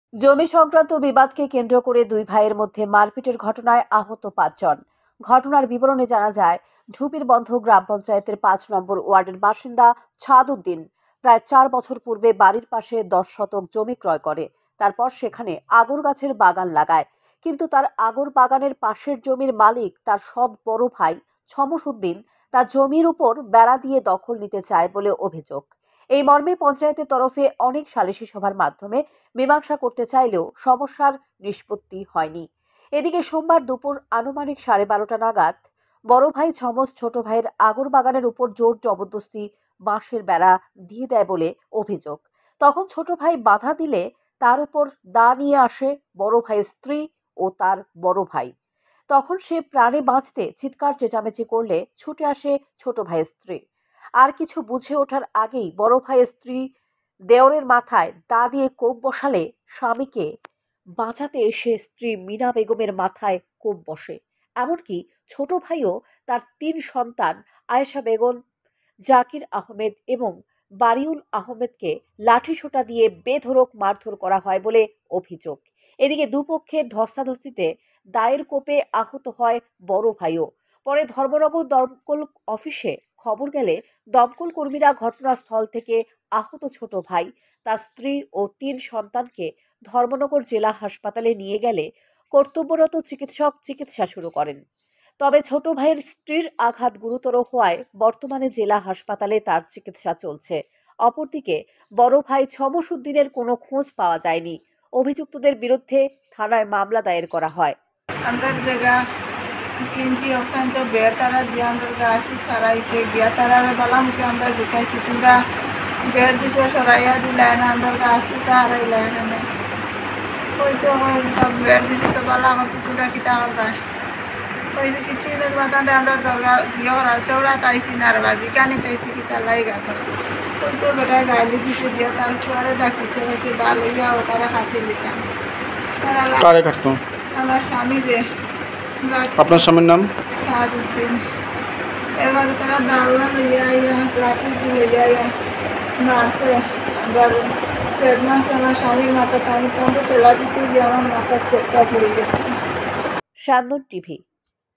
দুই ভাইয়ের মধ্যে মারপিটের ঘটনায় আহত পাঁচজন।
দুই ভাইয়ের মধ্যে মারপিটের ঘটনায় আহত পাঁচজন। ঘটনার বিবরণে জানা যায়, ঢুপিরবন্ধ গ্রাম পঞ্চায়েতের পাঁচ নং ওয়ার্ড এলাকায়